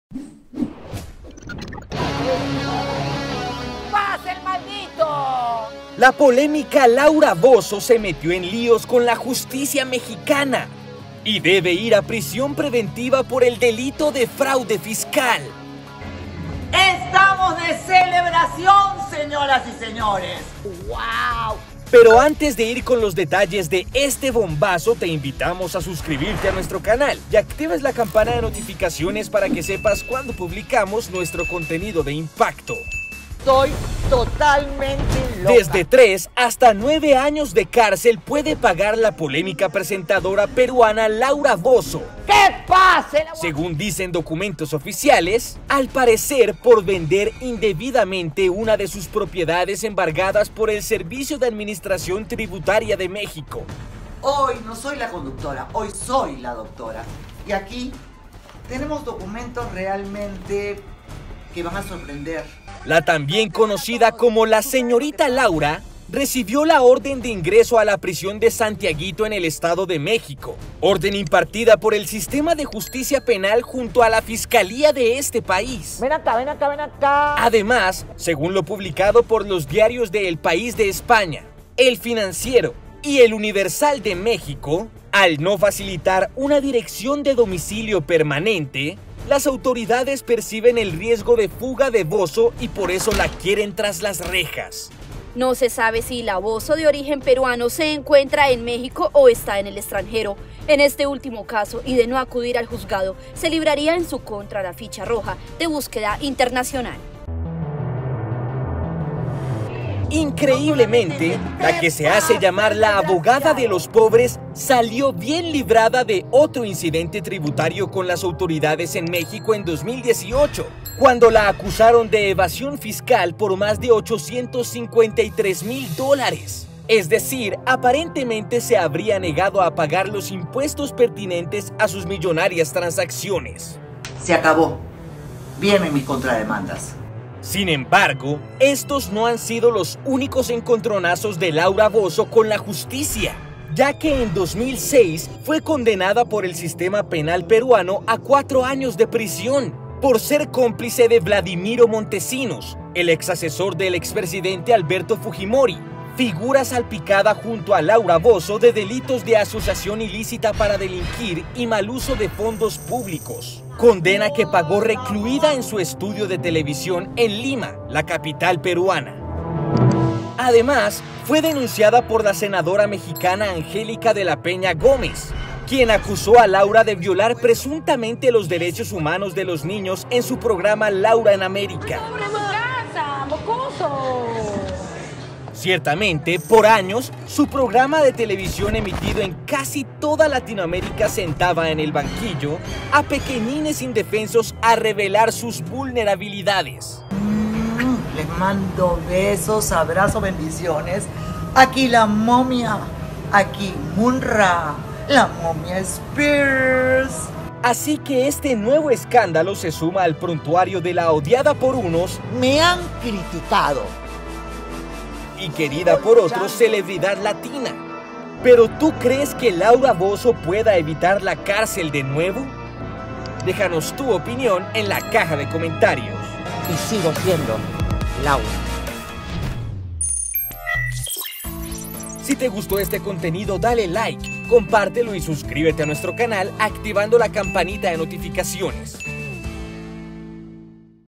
LAURA BOZZO a la CÁRCEL por ¡FRAUDE FISCAL en MEXICO! | Impacto Mundo
La polémica presentadora y conductora Laura Bozzo deberá ir a prisión preventiva en México por un nuevo escándalo de posible fraude fiscal que mancha y daña su reputación. En Impacto Mundo investigamos y presentamos todos los detalles. Periodista: Jorge Barrera